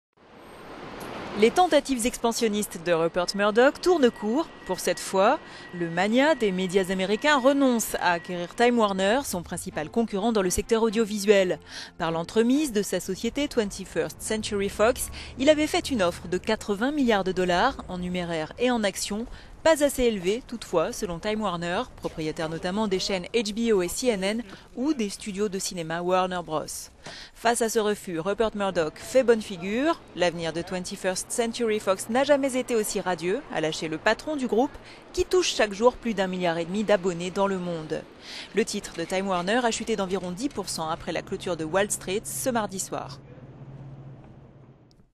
Rupert Murdoch renonce à acquérir Time Warner - economy
Les tentatives expansionnistes de Rupert Murdoch tournent court... pour cette fois.

Le magnat des médias n'aura pas Time Warner, son principal concurrent dans le secteur audiovisuel américain.
Par l'entremise de sa société 21st Century Fox, il avait fait une offre de 80 milliards de dollars, en  numéraire et en actions.
"Pas assez élevée" a toutefois estimé Time Warner, propriétaire notamment des chaîne HBO et CNN ou des studios de cinéma Warner Bros.